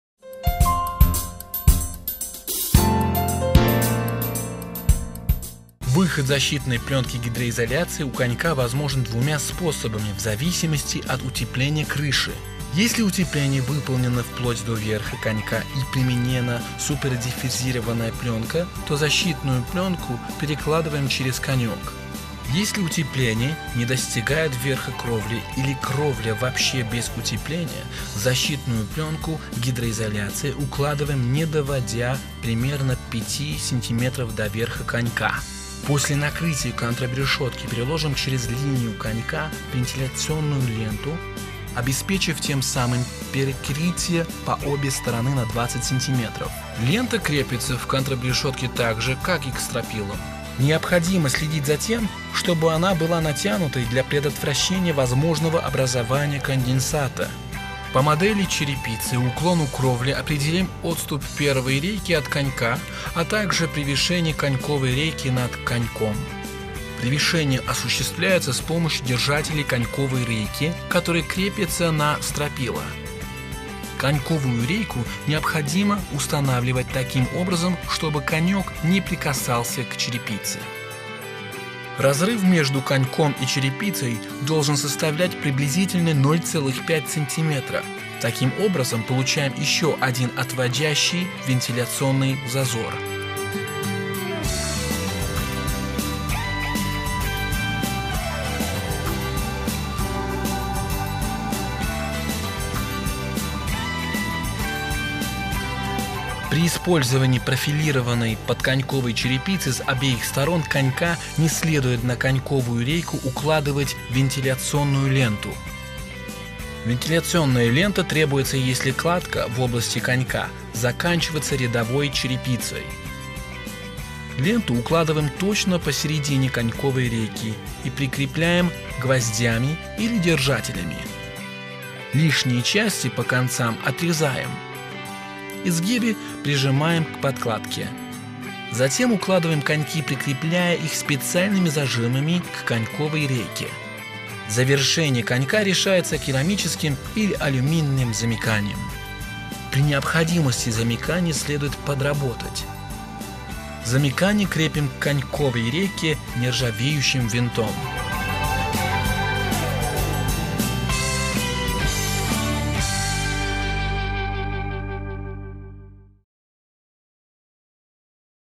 Монтаж керамической черепицы TONDACH Устройство конька кровли
Инструкция по монтажу керамической черепицы TONDACH. Устройство кровельного конька.